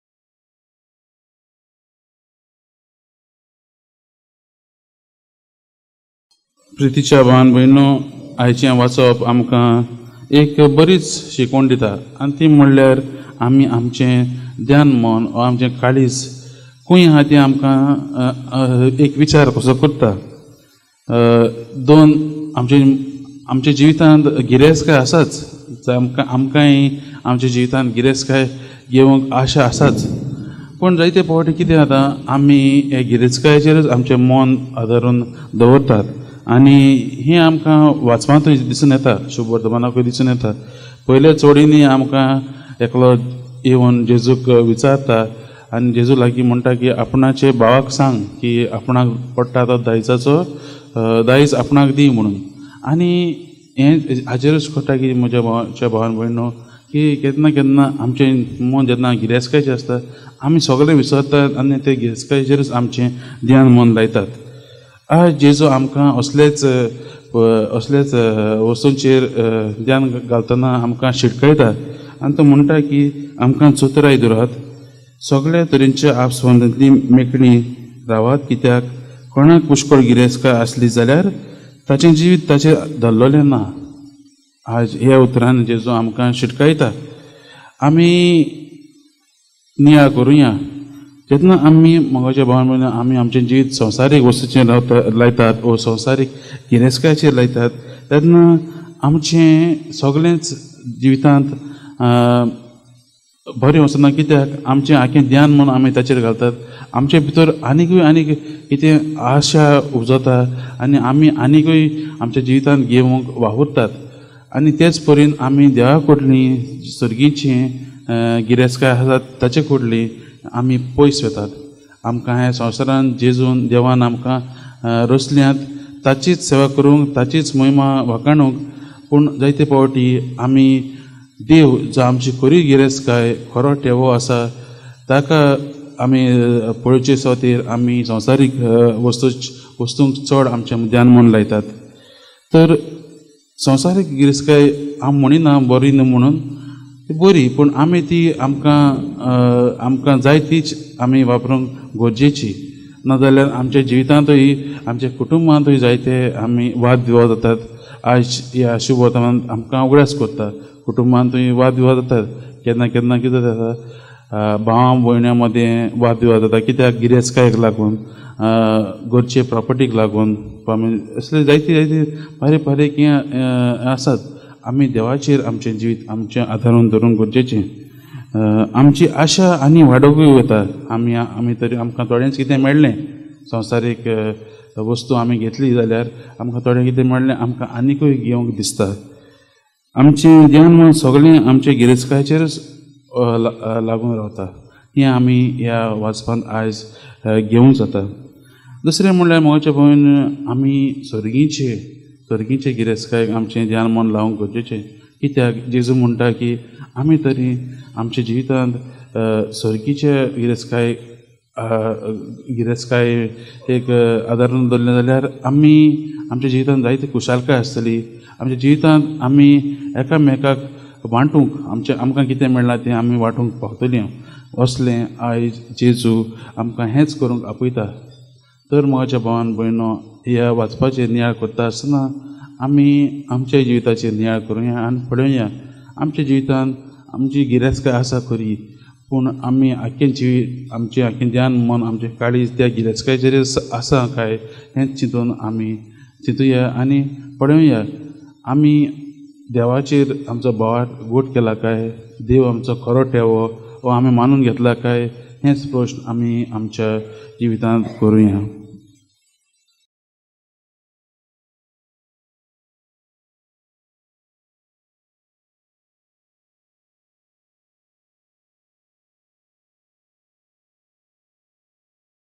Bhavarth Gott Korat🙏 Homily in Konkani 🙏 21 Oct 2024
The reflection on the readings of the day by Fr Shawn Dsilva SJ